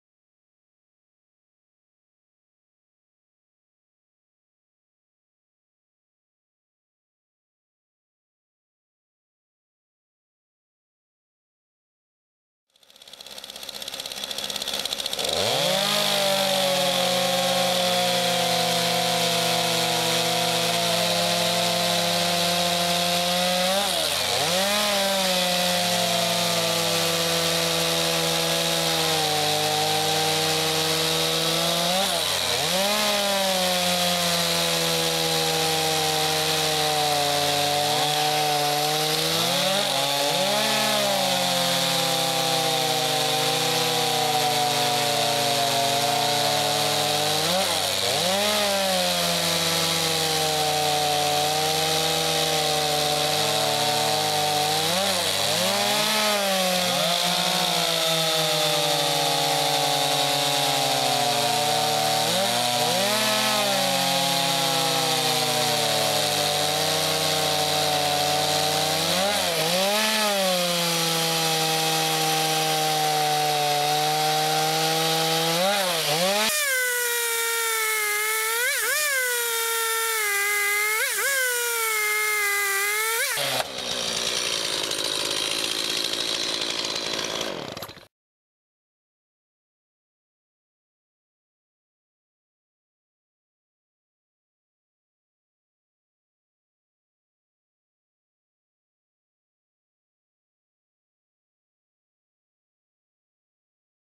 STIHL Picco Duro vs. Conventional Chain - English
A field demonstration of STIHL Picco Duro chain vs. a conventional chain. STIHL Picco Duro chain stays sharp up to four times longer than conventional chain. Find it today at you local STIHL Dealer to find the dealer nearest to you.